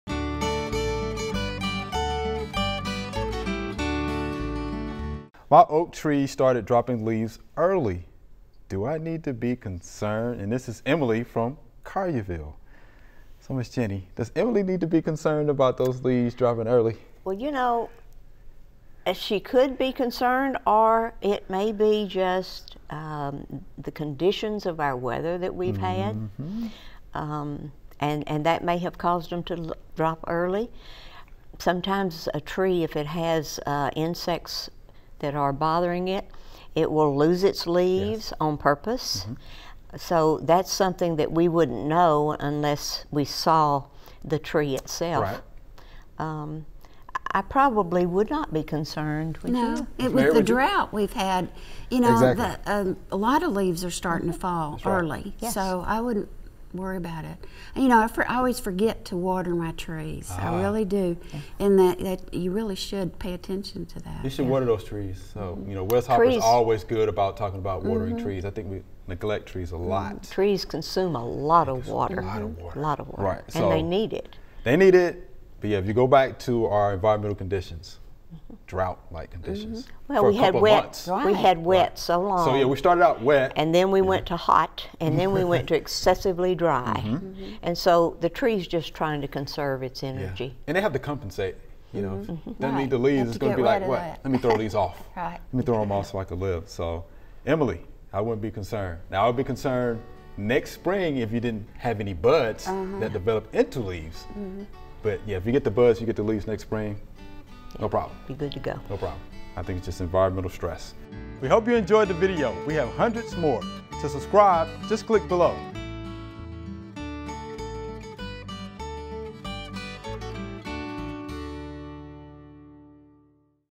Q&A – Do I need to be concerned that my oak tree started dropping leaves early?
You don’t need to be worried now. The last half of this summer was very dry, and the oak trees are dropping their leaves early as a way to preserve energy and water. Trees may also throw their leaves early to defend against an insect infestation. In both cases it is not something to be concerned about. It will probably be okay. If the tree does not get buds and leaves next year then something happened.